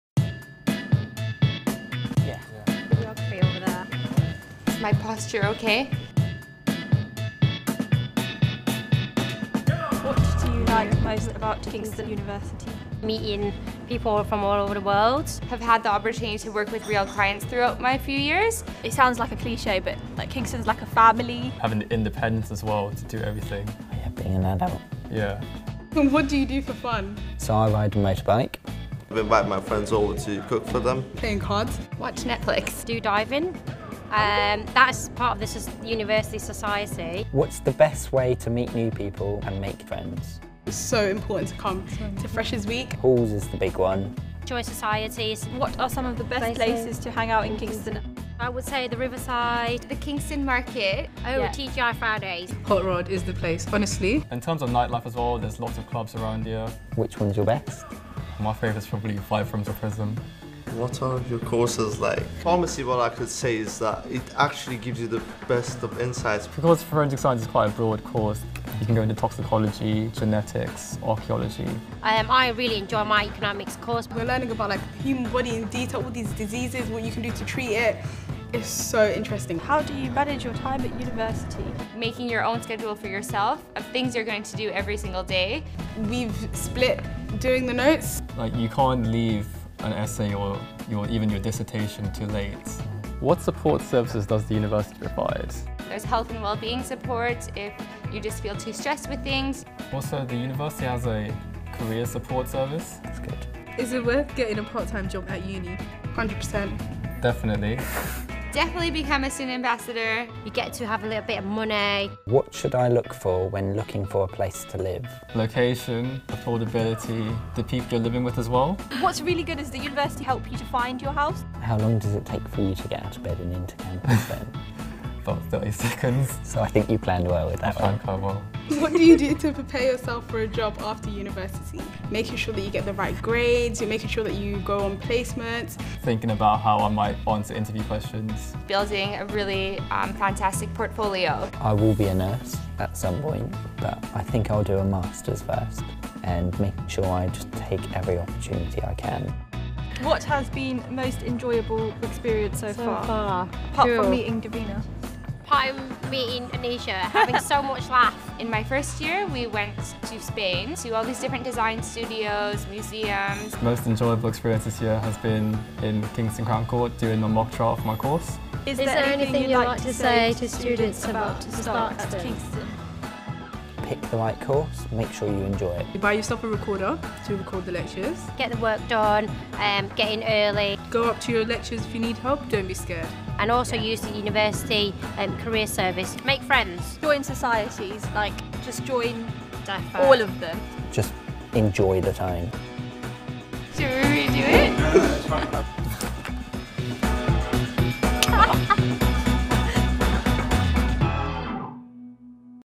What is Kingston University really like? | Student Q&A
Our students answer some of the most frequently asked questions by those about to start at university.